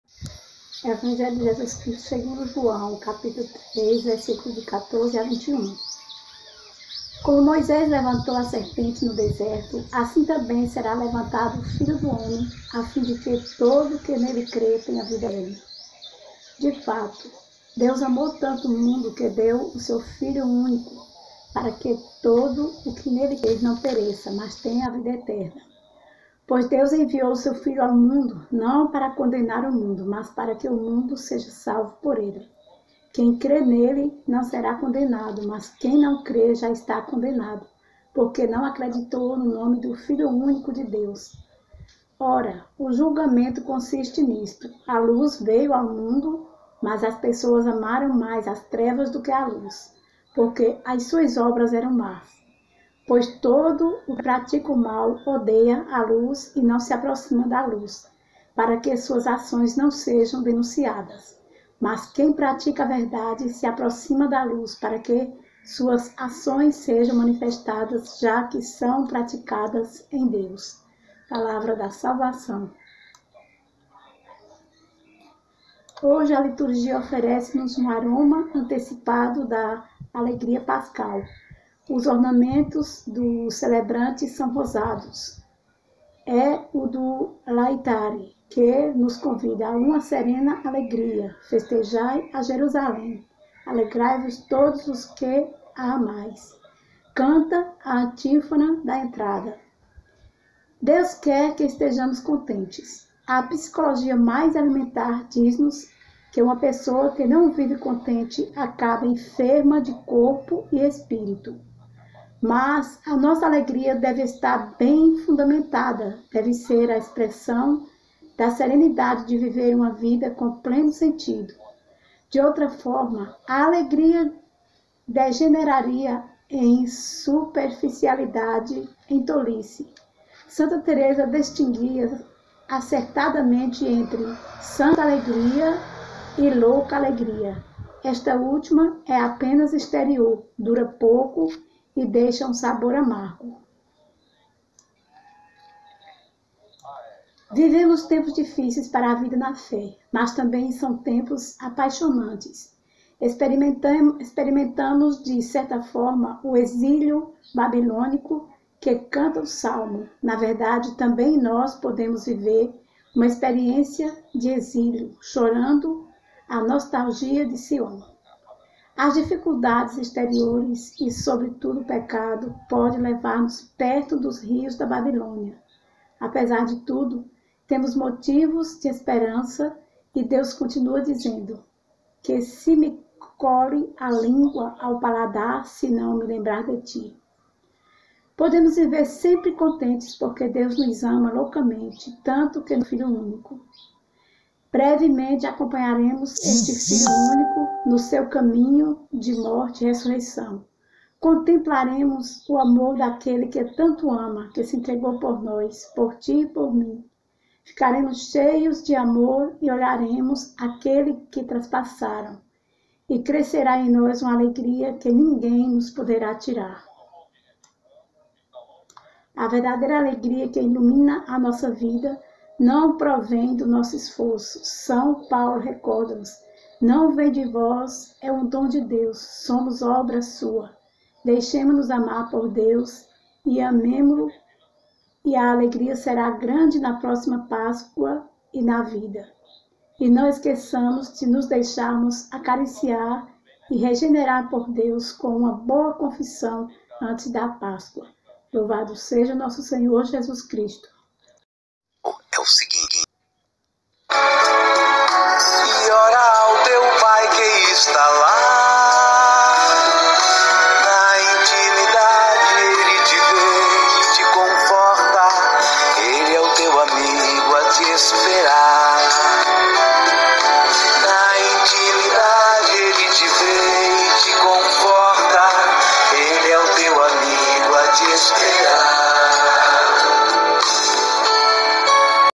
Evangelho (Jo 3,14-21):
lDeus quer que estejamos contentes. A psicologia mais elementar diz-nos que uma pessoa que não vive contente acaba enferma de corpo e espírito. Mas, a nossa alegria deve estar bem fundamentada, deve ser a expressão da serenidade de viver uma vida com pleno sentido. De outra forma, a alegria degeneraria em superficialidade, em tolice. Santa Teresa distinguia acertadamente entre “santa alegria” e “louca alegria”. Esta última é apenas exterior, dura pouco e deixa um sabor amargo.